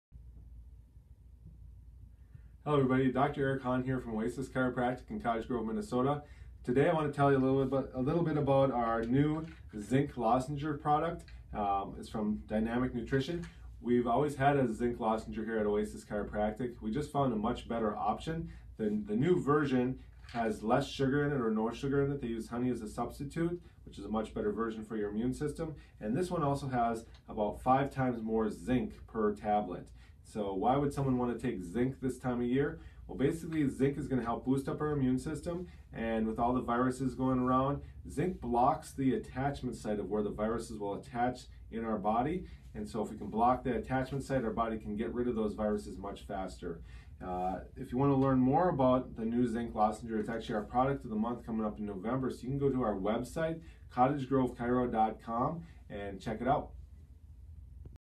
Cottage Grove Chiro, Dr Eric discusses Zinc supplement
Find out why you need this Zinc supplement during cold season with Dr Eric Hahn